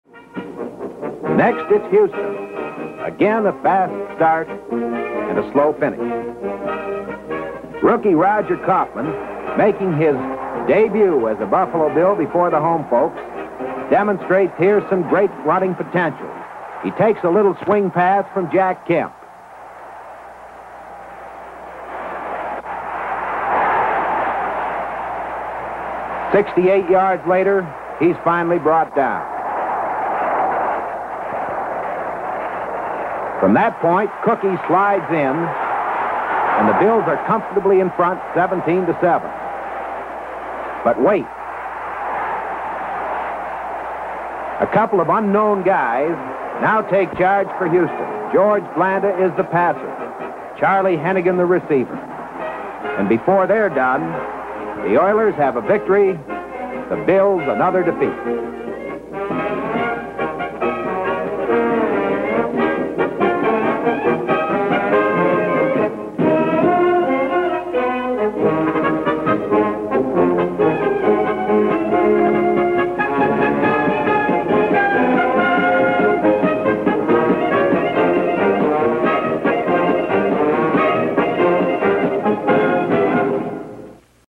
Sept. 28, 1963 | Bills vs. Oilers highlights
Sept. 28, 1963 - Here are some highlights of today's game between the Buffalo Bills and Houston Oilers at War Memorial Stadium in Buffalo. The Oilers won the AFL contest, 31-20.